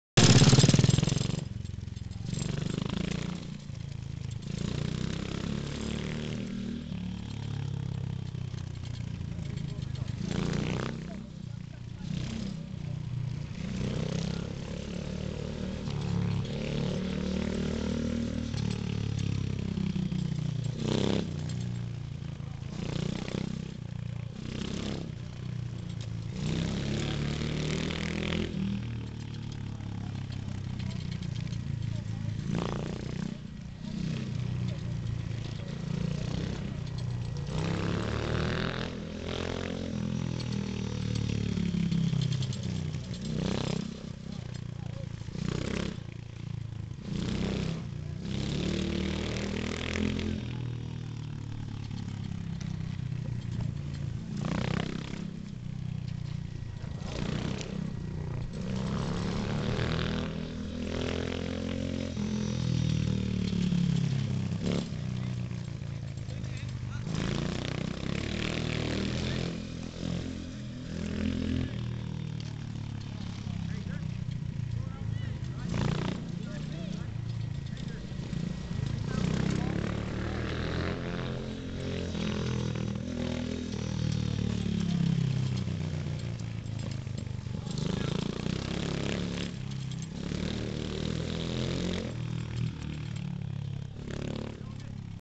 me doing speedway
me having a spin round northside on a 140cc bike